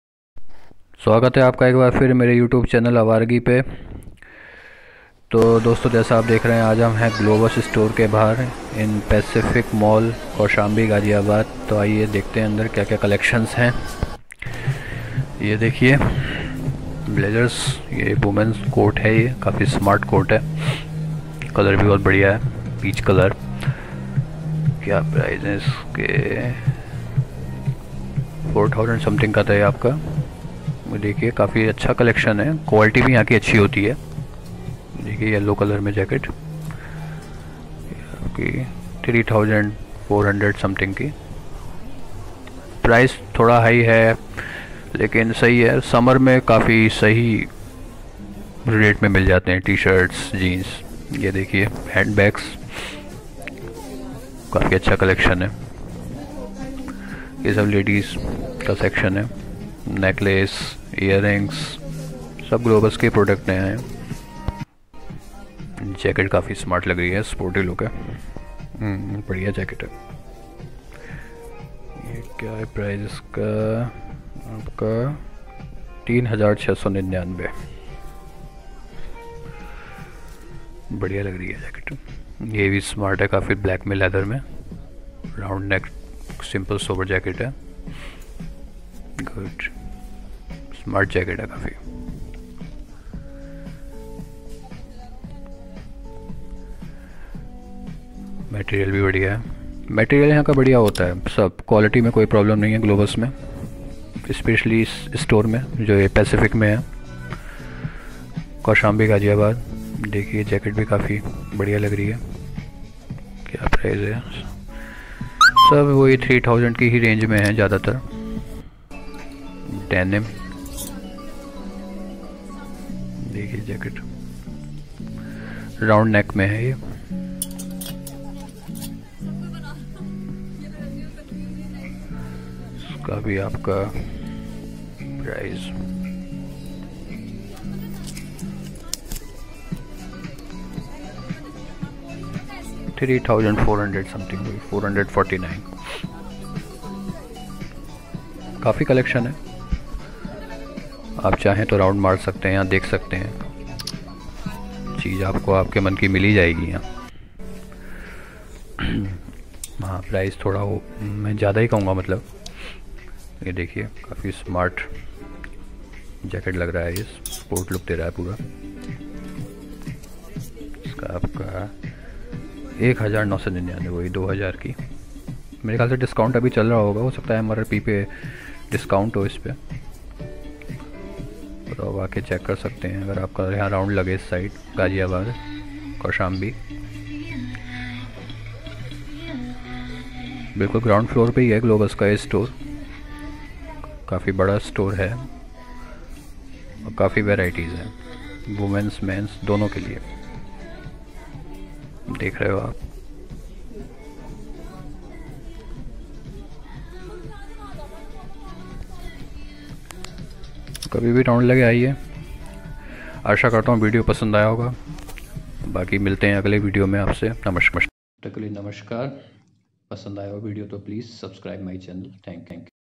Inside Globus Store Pacific Mall Kaushambi Ghaziabad,UP
Globus is a fashion accessories clothing brand where you will find a large collection of clothes,artificial jewelry,hand bag's etc.Today we are Globus store in Pacific Kaushambi Ghaziabad.Let us try to show you the collection here through this video.